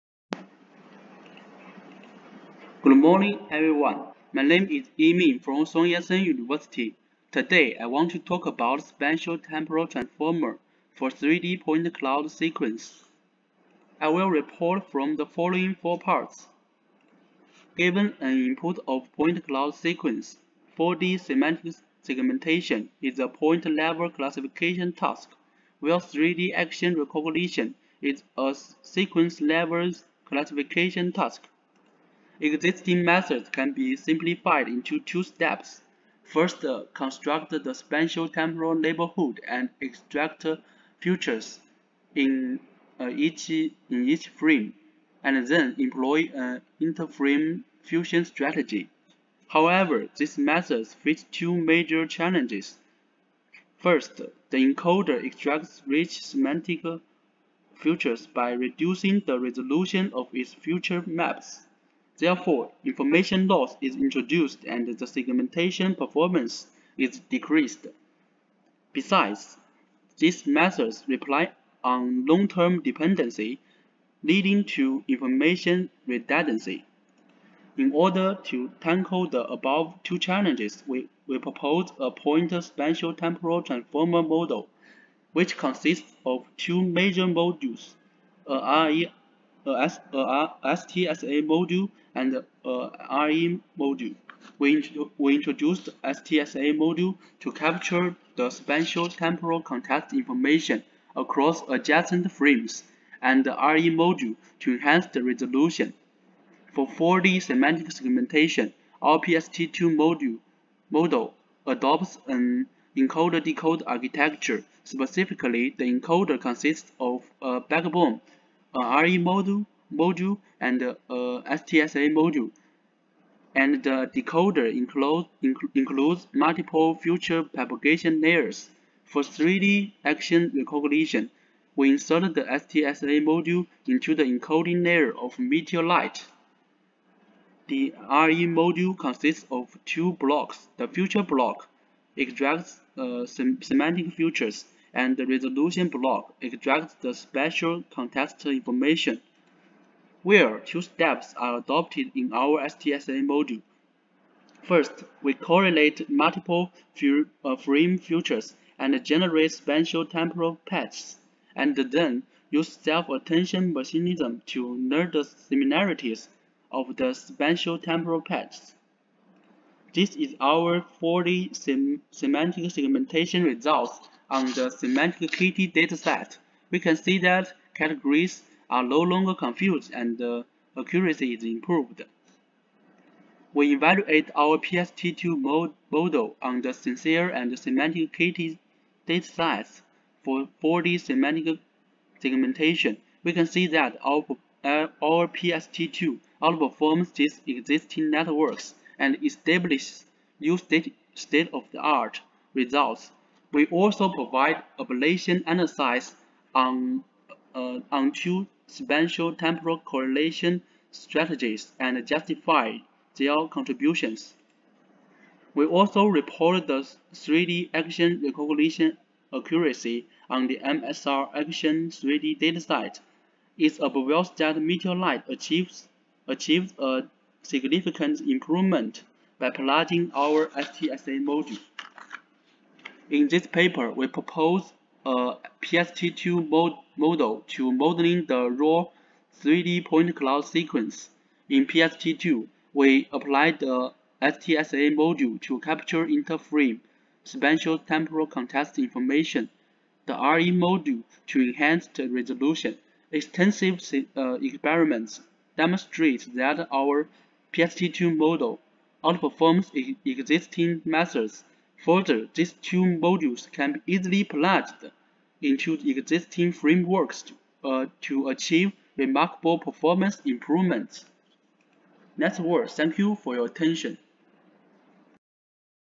Spatial-Temporal Transformer for 3D Point Cloud Sequences
Authors: Yimin Wei (Sun Yat-Sen University); Hao Liu (Sun Yat-Sen University); Tingting Xie (Queen Mary University of London); Qiuhong Ke (The University of Melbourne); Yulan Guo (Sun Yat-sen University)* Description: Effective learning of spatial-temporal information within a point cloud sequence is highly important for many down-stream tasks such as 4D semantic segmentation and 3D action recognition. In this paper, we propose a novel framework named Point Spatial-Temporal Transformer (PST2) to learn spatial-temporal representations from dynamic 3D point cloud sequences. Our PST2 consists of two major modules: a Spatio-Temporal Self-Attention (STSA) module and a Resolution Embedding (RE) module. Our STSA module is introduced to capture the spatial-temporal context information across adjacent frames, while the RE module is proposed to aggregate features across neighbors to enhance the resolution of feature maps. We test the effectiveness our PST2 with two different tasks on point cloud sequences, i.e., 4D semantic segmentation and 3D action recognition. Extensive experiments on three benchmarks show that our PST2 outperforms existing methods on all datasets. The effectiveness of our STSA and RE modules have also been justified with ablation experiments.